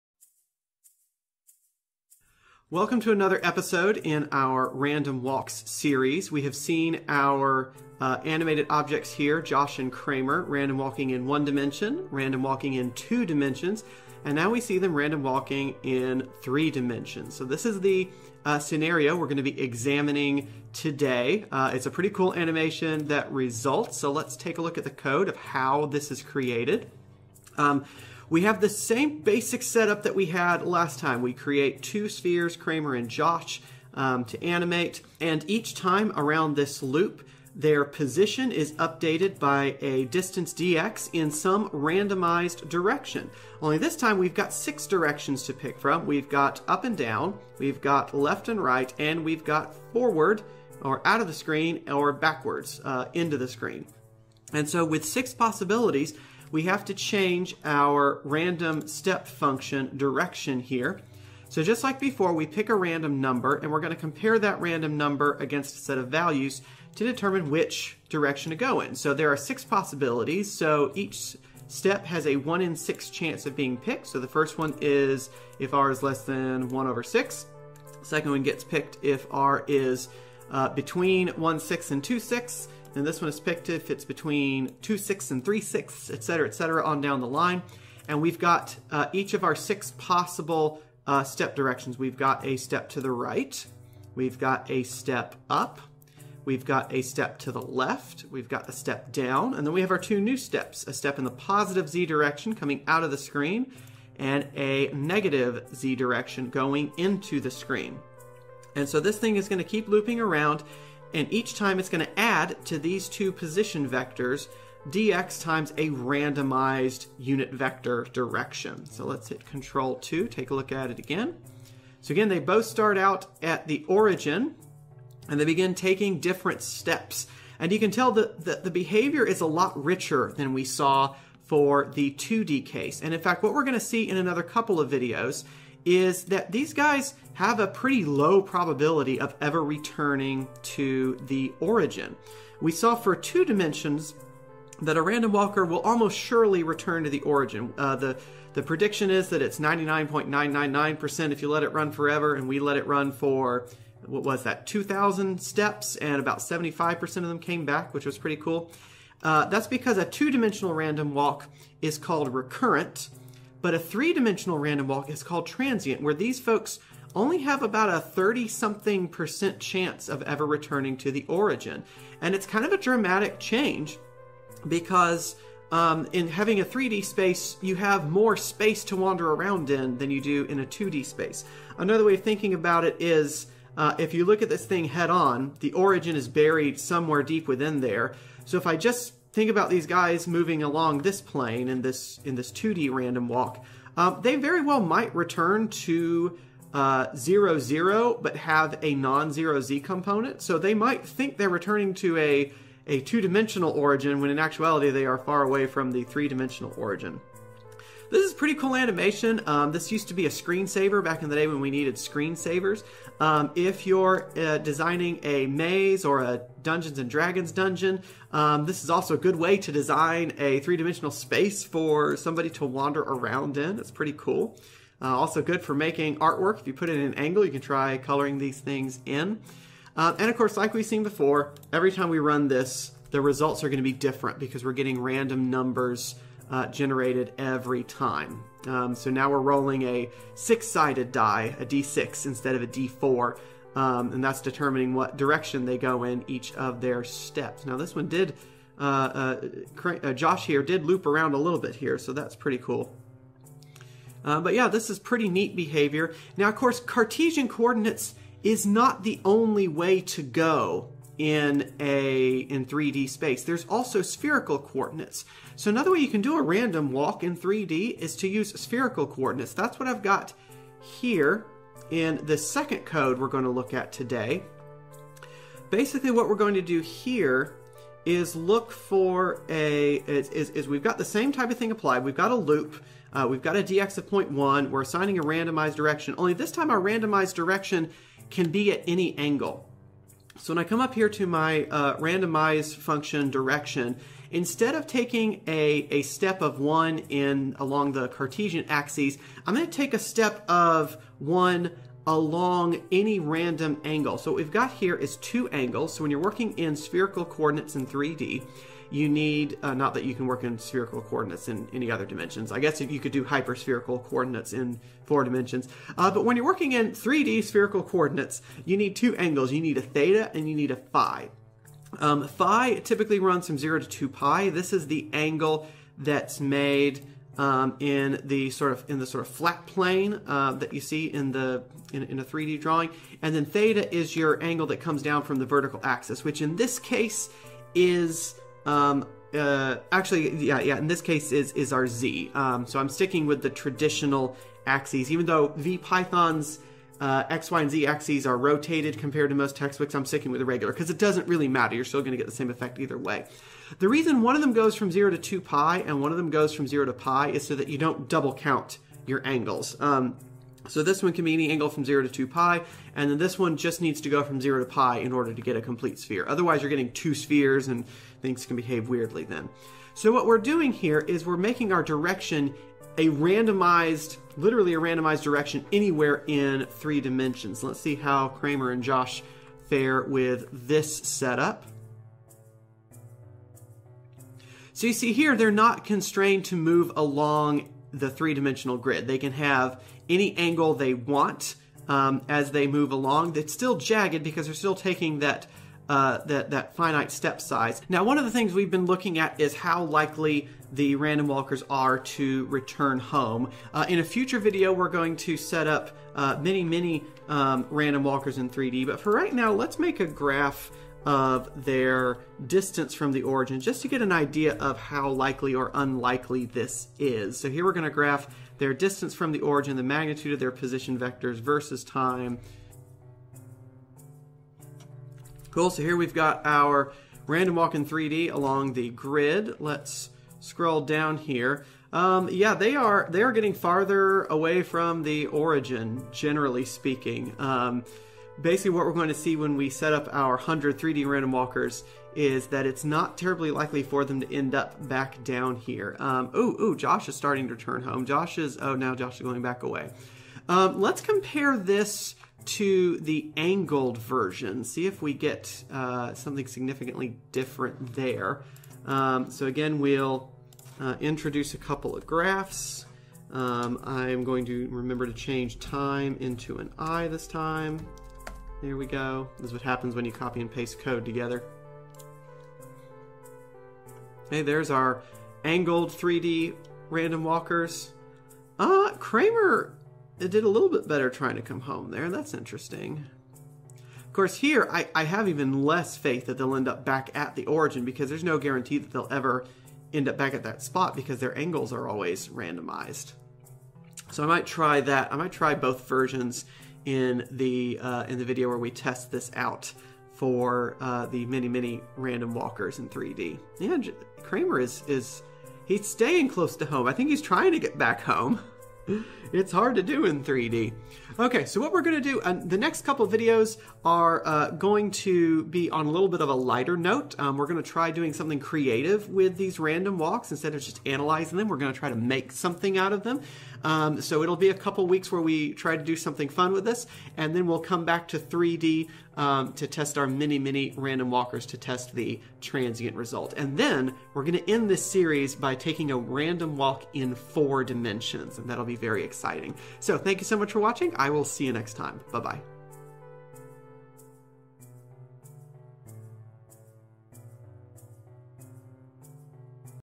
Random Walks 7 - Random Walk in 3-D
The random walk is a great tool for modeling randomly driven processes.